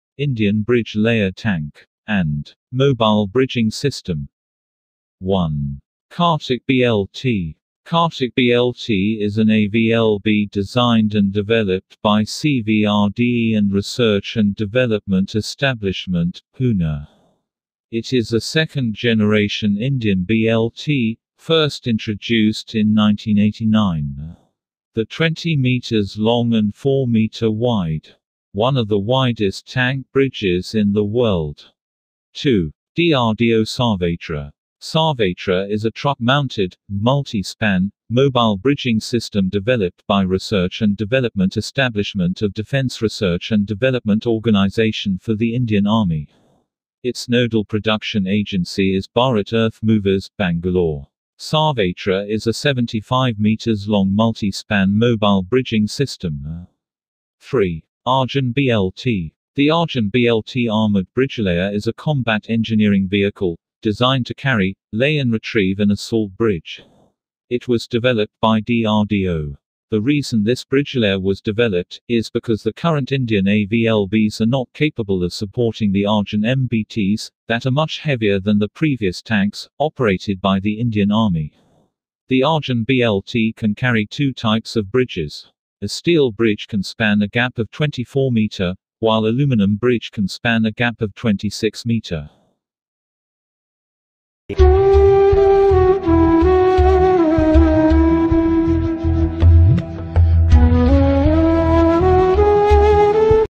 Indian Bridge Layer Tank, Mobile Bridging System, AVLB. Kartik BLT, DRDO Sarvatra, Arjun BLT.
Indian army use this bridges. Kartik BLT, DRDO Sarvatra, Arjun BLT. Arjun BLT is specially designed and developed for arjun MBT.

About Channel : ArmedForce channel on YouTube where you will find news update, technology and video related to the indian defence and space.